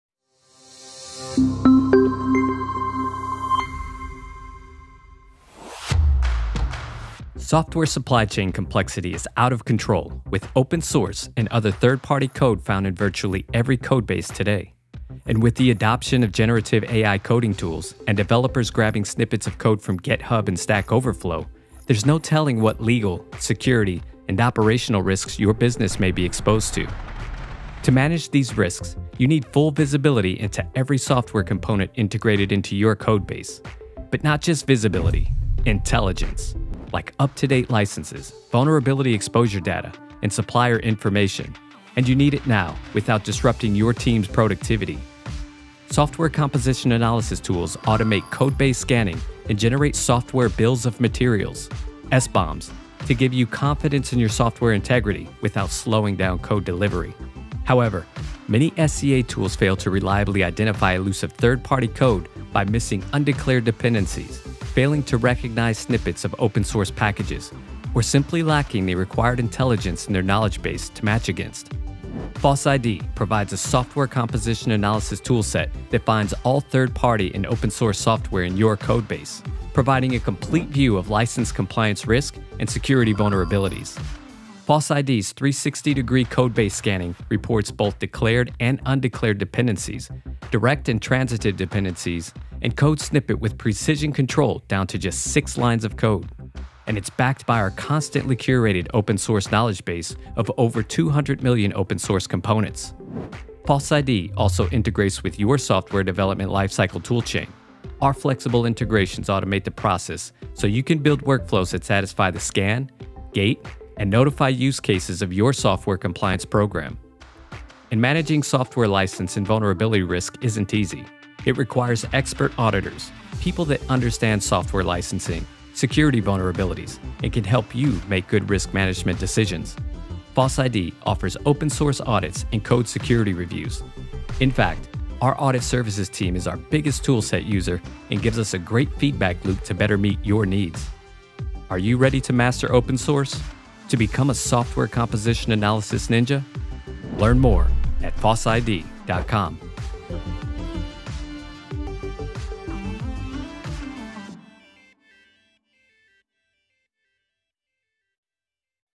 Master Open Source: Navigate Software Composition Analysis with FossID
Navigate open source complexities with FossID’s software composition analysis tools. Ensure visibility, manage risks, and maintain productivity. Master SCA with FossID.